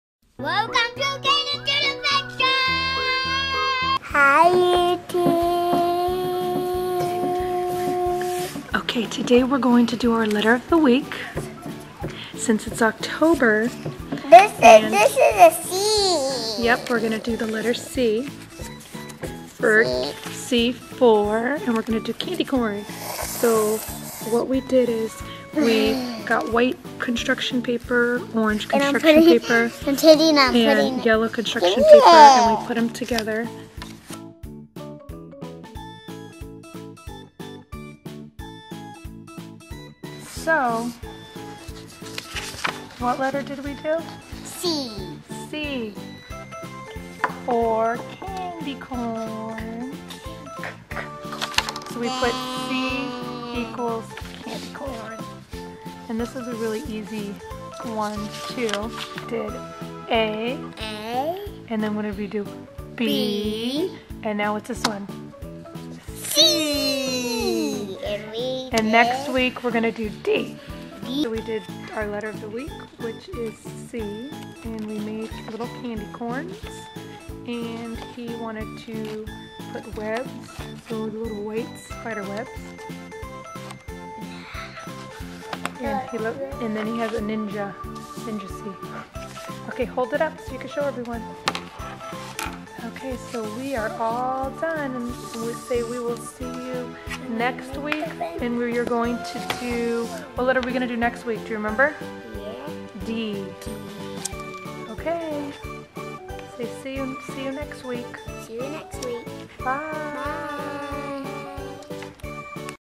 Kids making Fall Crafts! Candy Corn Letter C! October 2018!
Caden's letter of the week is C!! He made Candy Corns & a big letter C!